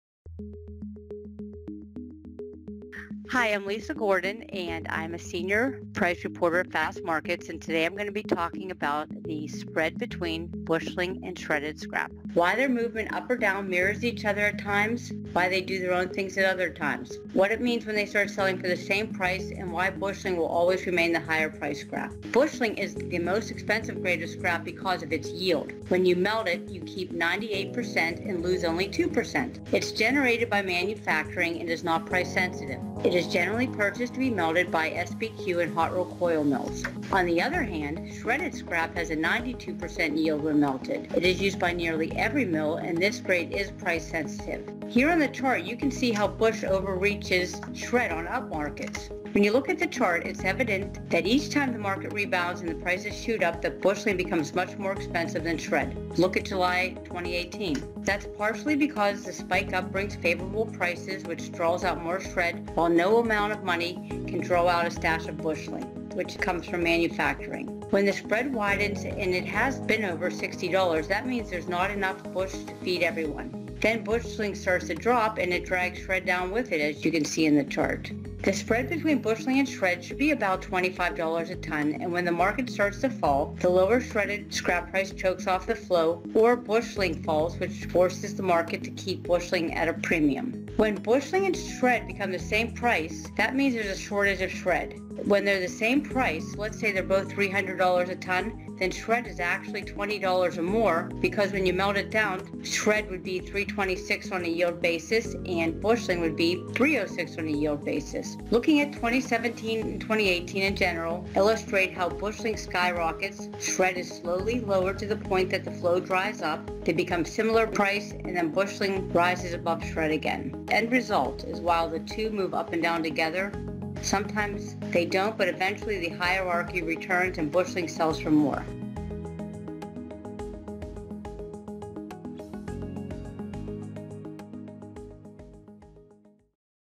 Fastmarkets Shorts: Understanding the spread between busheling and shredded scrap
Lisa Gordon, senior price reporter for the US ferrous scrap market, discusses the relationship between No1 busheling and shredded scrap and how that impacts the spread between prices.